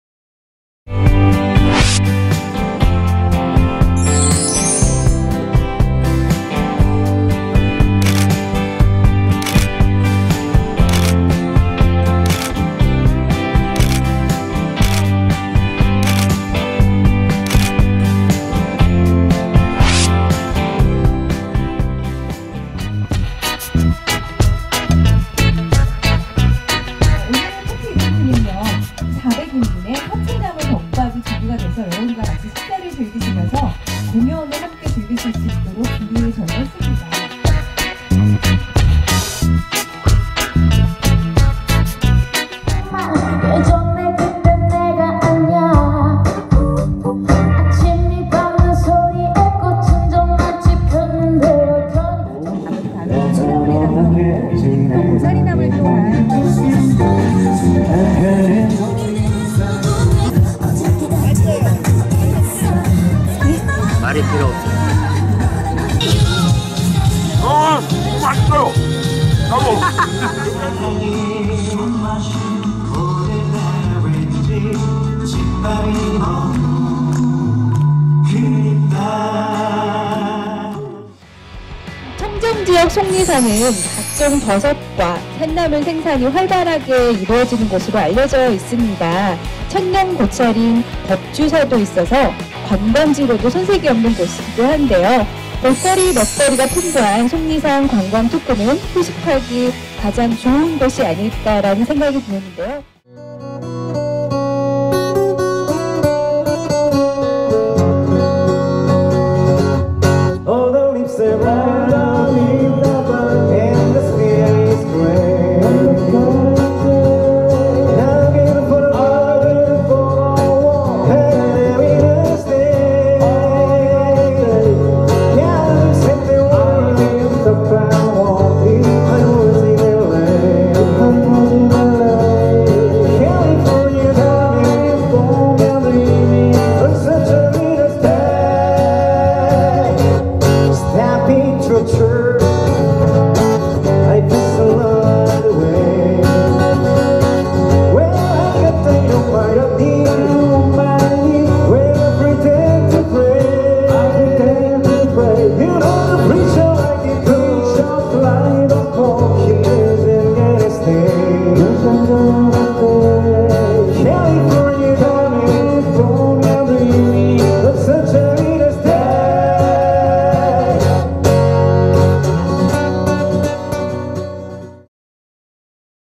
보은속리산 감동밥상 파티 이벤트 korea food festival 엔케이엔뉴스
속리산관광협의회가 주측이 되어 이 지역의 특산물로 새로운 음식을 만들어 보고, 함께 나누고...

한국문화 축제을 보시려면 좋아요, 구독신청 바랍니다... . 엔케이엔뉴스